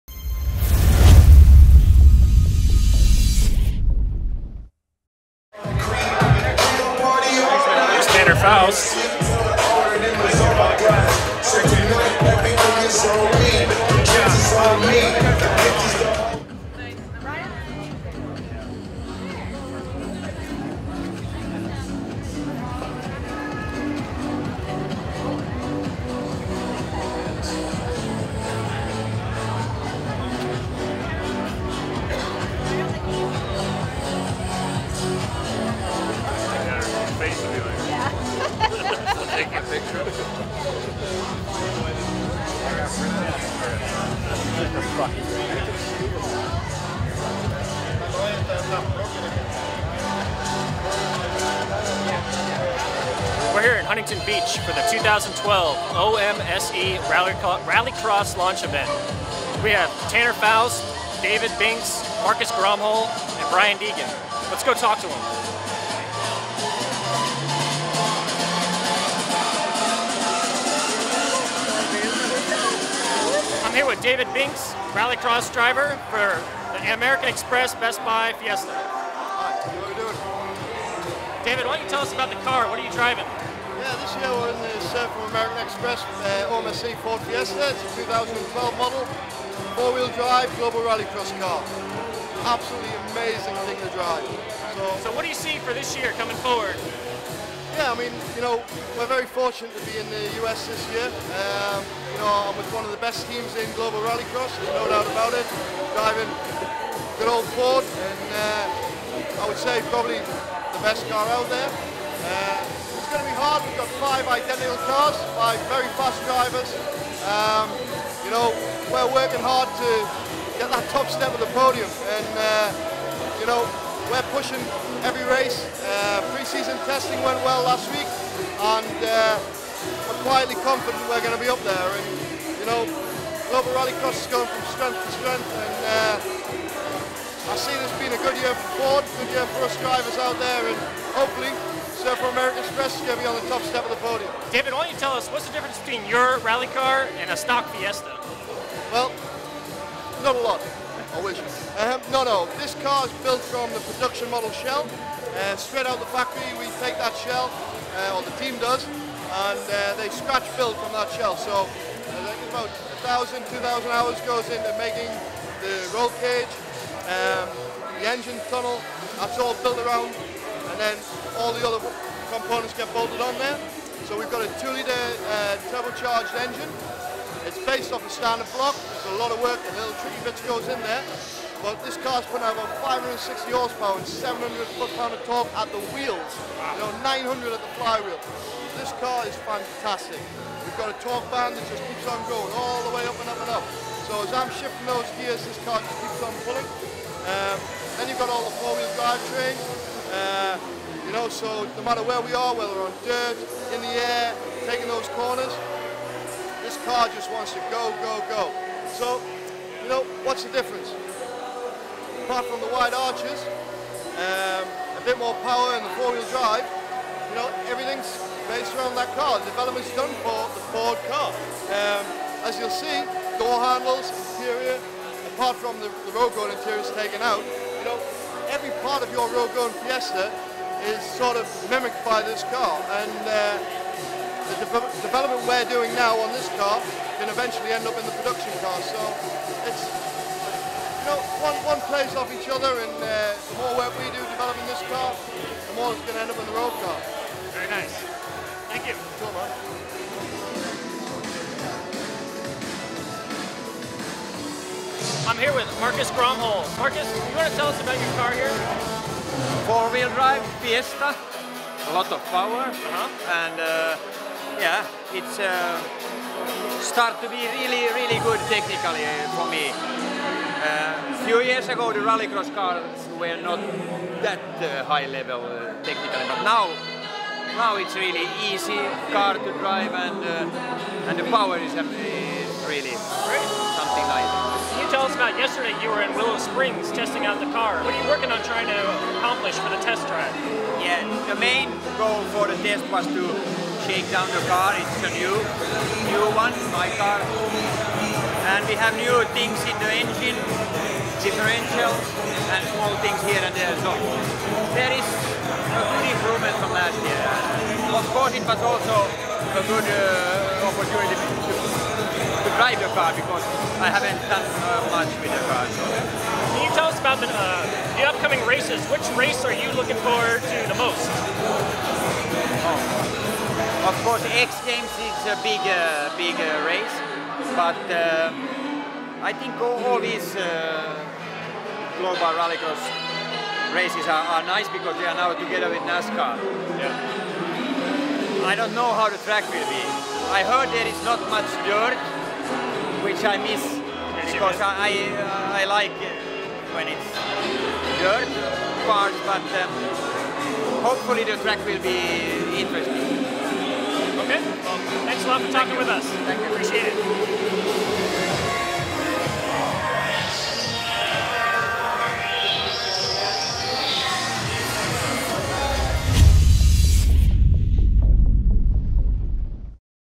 Ford/OMSE RallyCross Launch Party 2012
We were invited to attend this OMSE event announcing their five drivers for Global Rallycross Championship - Tanner Foust, Brian Deegan, Marcus Gronholm, David Binks and Toomas Heikkinen.